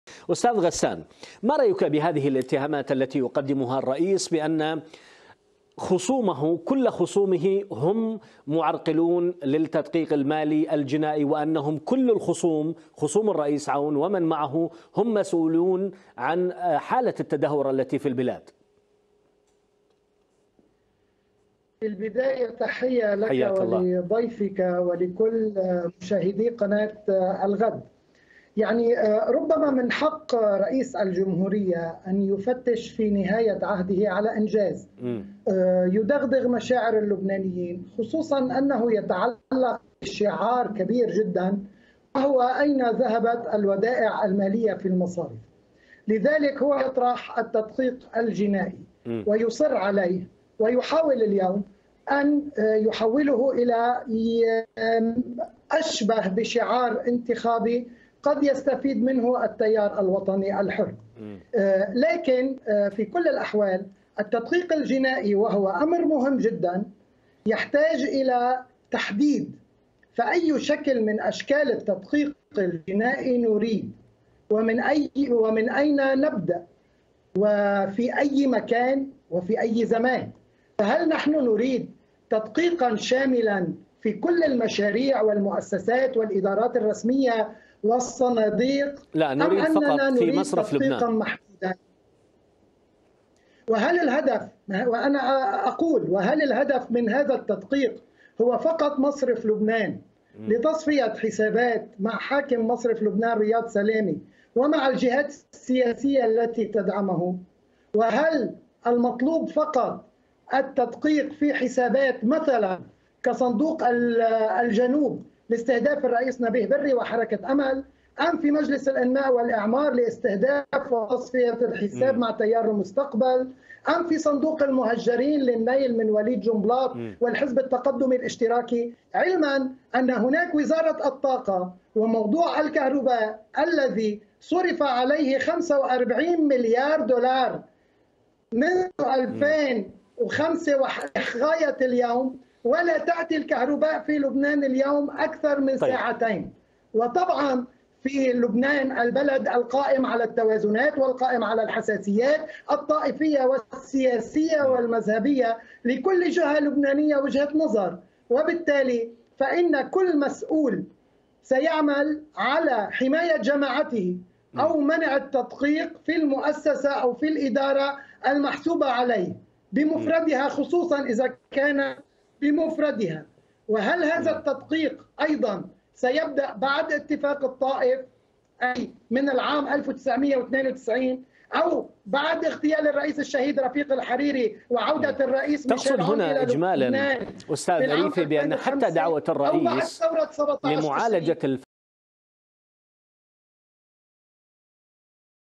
صحفي: عون يستخدم "التدقيق الجنائي" كشعار انتخابي لدغدغة مشاعر اللبنانيين
اتهم الرئيس اللبناني ميشال عون خصومه بمحاولة إجهاض التدقيق الجنائي المالي «والحؤول دون وصوله إلى النتائج المرجوة التي تلقي الضوء على الأسباب الحقيقية التي أوصلت البلاد إلى هذا المنحدر وتحاسب المسؤولين عن ذلك.
يناقش هذا الموضوع من بيروت مالك أبي نادر القيادي في التيار الوطني الحر، ومن بيروت غسان ريفي رئيس تحرير جريدة سفير الشمال، مع الإعلامي علاء عبود خلال برنامج